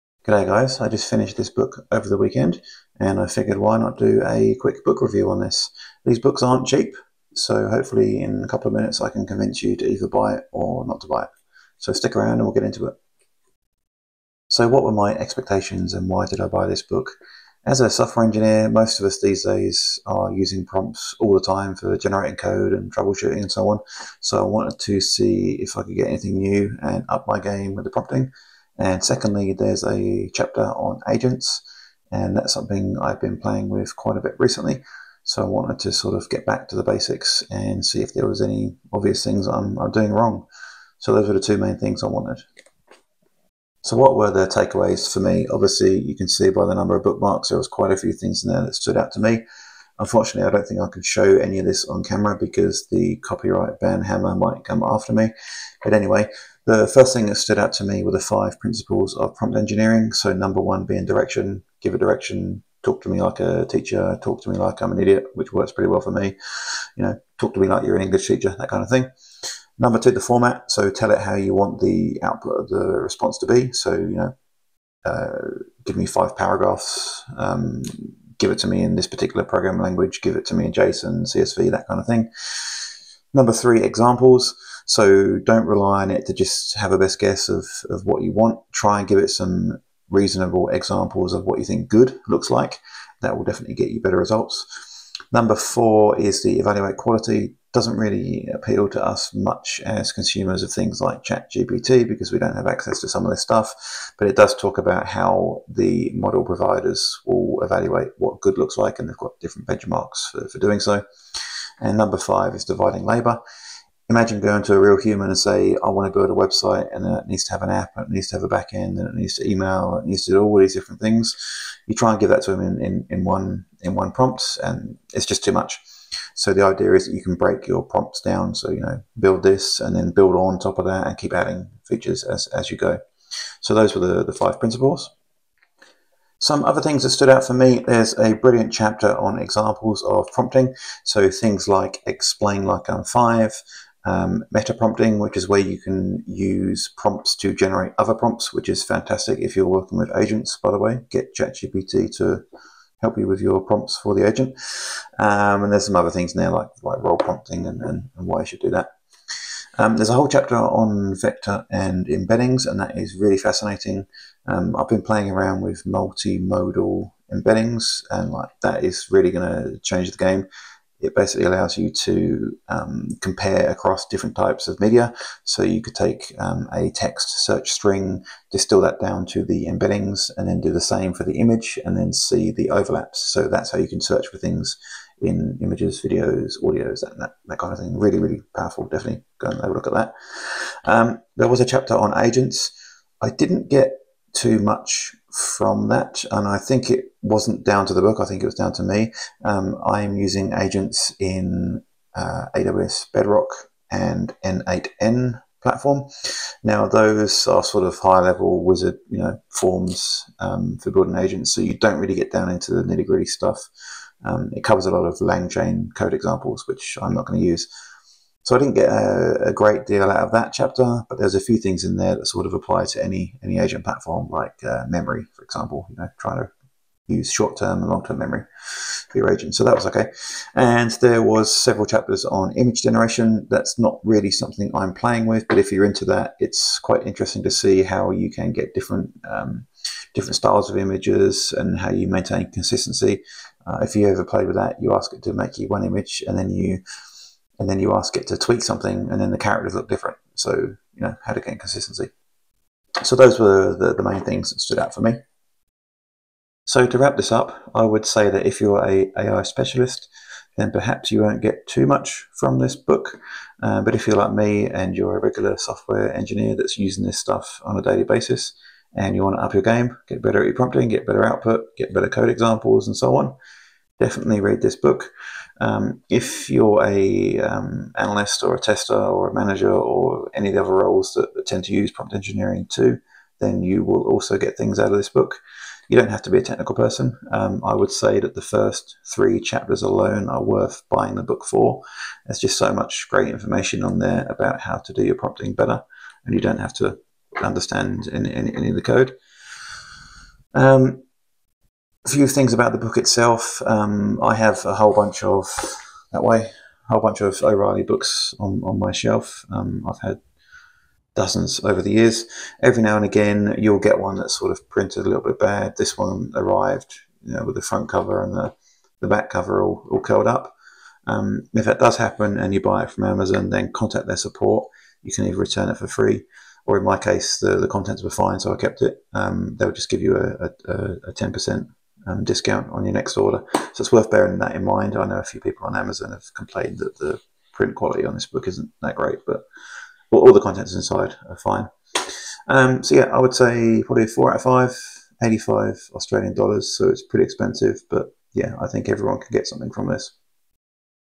Prompt Engineering for GenAI – 6-Minute Review of O’Reilly’s Essential Guide
In this quick 6-minute video, I review O’Reilly’s “Prompt Engineering for Generative AI”, a practical guide for anyone looking to write better prompts for LLMs like GPT-4, Claude, and Gemini.

I cover:
* What I expected from the book
* My key take aways from the book
* Whether it's worth your time

If you’re working with ChatGPT, Claude, or any GenAI tools, this book might help level up your prompting game — or it might just tell you what you already know. Find out which!

Chapters:
00:00 - Intro
00:14 - Expectations
00:43 - Take aways
04:17 - Wrap up

📖 Book: Prompt Engineering for Generative AI by O’Reilly Media